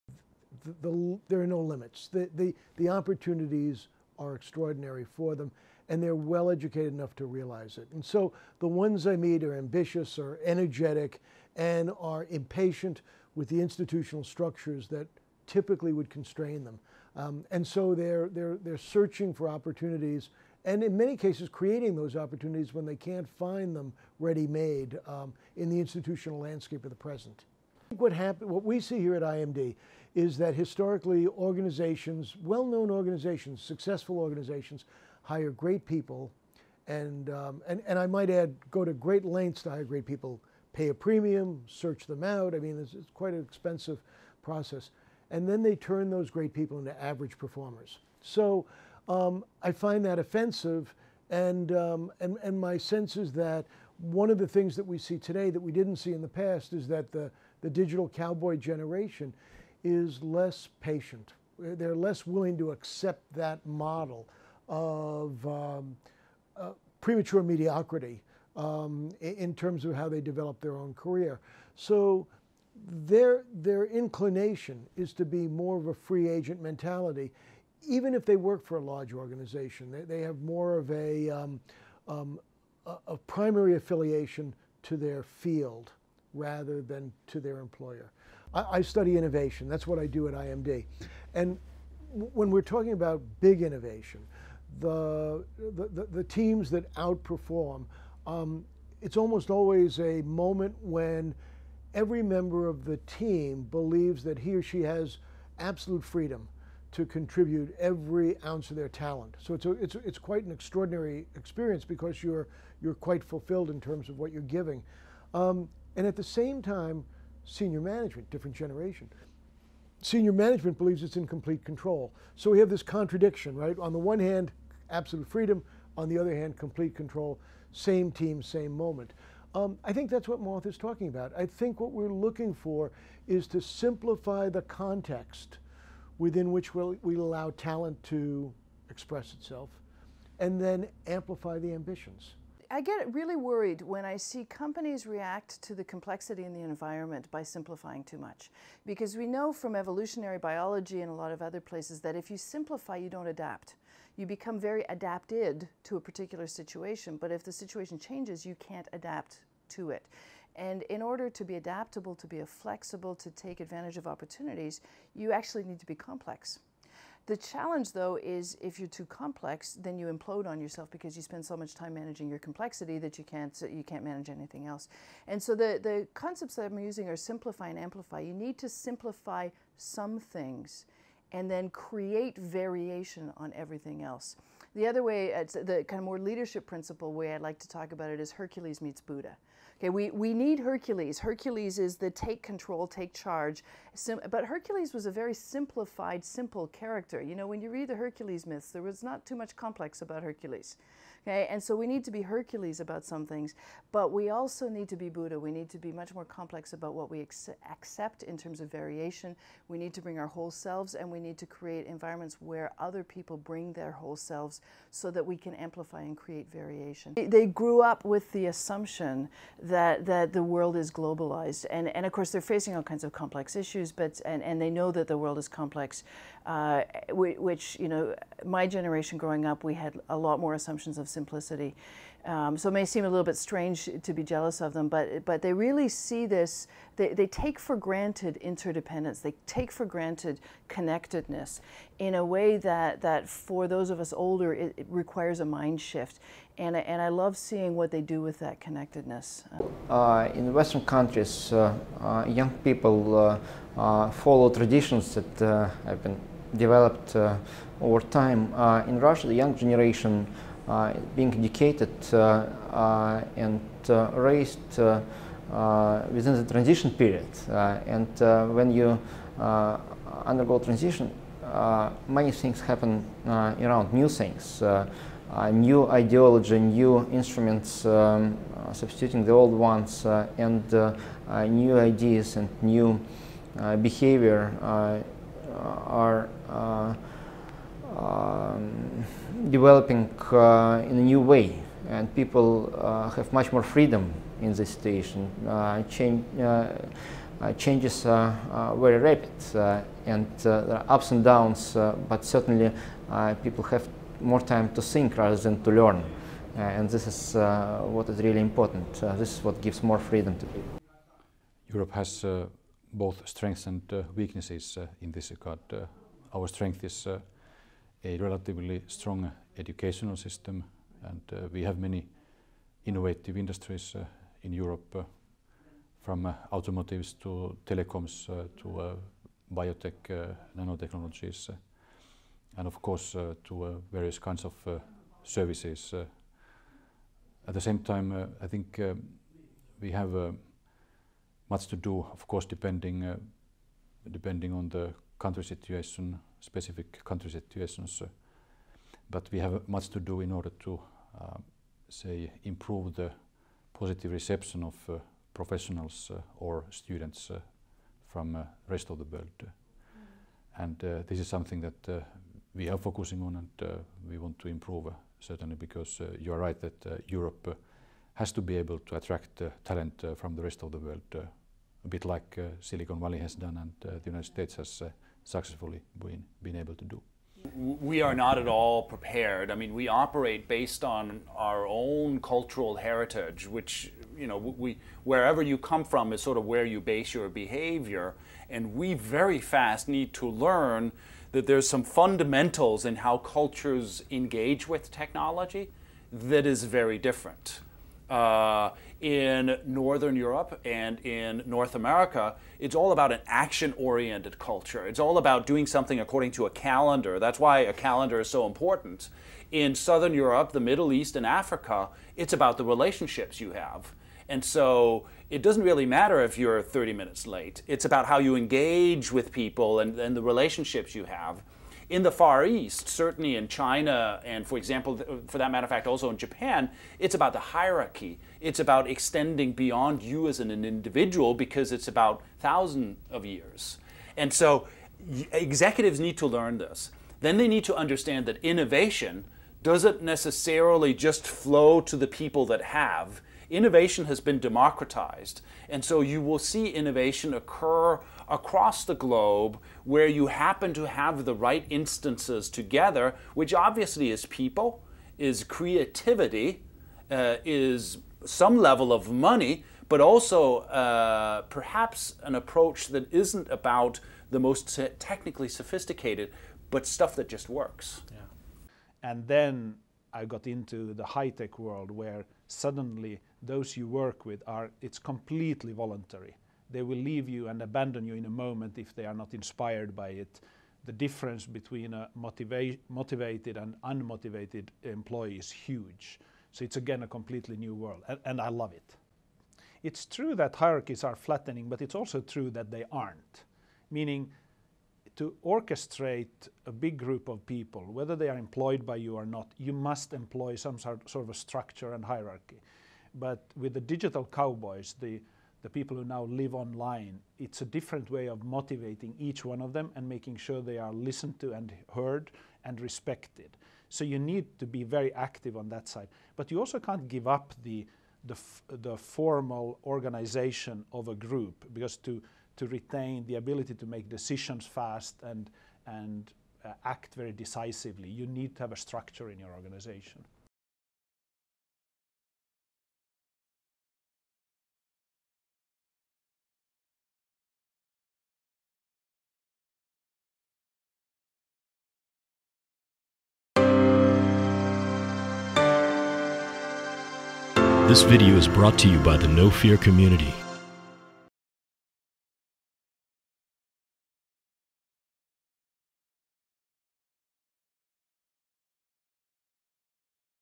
Full Commentary for No Fear...
A selection of guest speakers talk about the themes of new book 'No Fear' by Pekka A. Viljakainen.